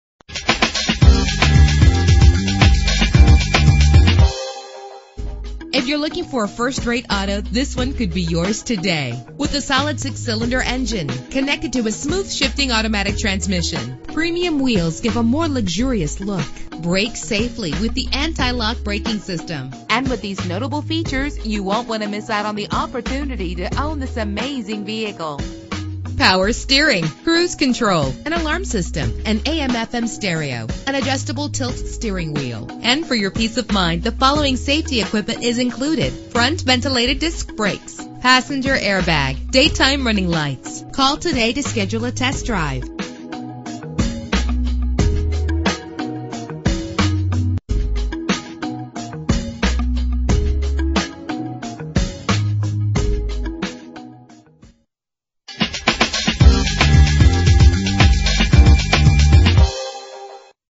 2000 Chevrolet Blazer - West Chicago IL
Year: 2000
Make: Chevrolet
Model: Blazer
Trim: LS
Engine: 4.3 liter 6 cylinder 12 valve
Transmission: 4-Speed Automatic
Color: Light Pewter Metallic
Mileage: 114026
Address:
601 W Roose